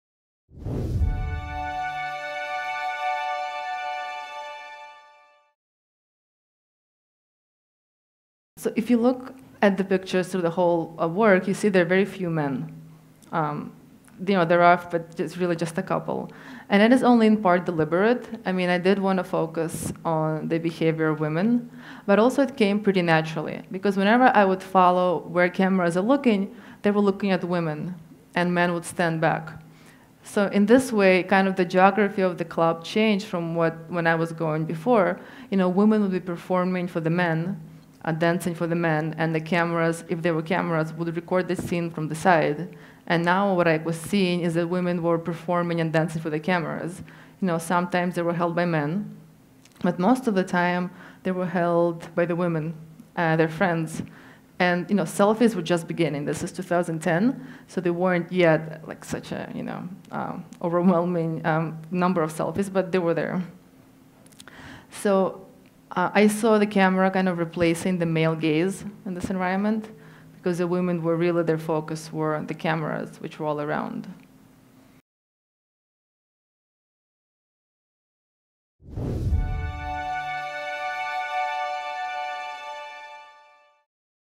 Dina Litovsky on Very Few Men
Photographer Dina Litovsky examines social performances, contemporary rituals and group interactions.
Interested?